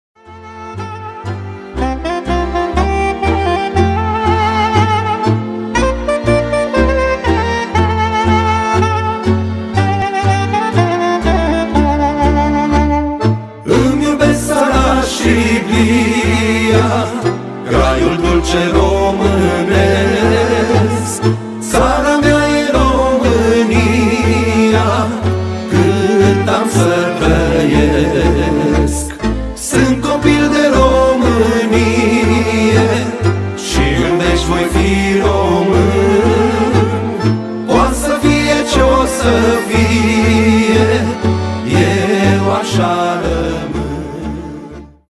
Invitatie la petrecere de Ziua Unirii la Pescarus, restaurant pentru nunta, botez si petreceri.
Petrecere de Ziua Unirii (24 ianuarie) cu formatia Rustic la Restaurant Pescarus, restaurant specializat in organizarea de petreceri private (zile de nastere, onomastici, nunta, botez), petreceri firme, petreceri publice (revelion, Ziua Femeii, Ziua Indragostitilor, etc.). Meniu obligatoriu. Vinul casei este la discretie!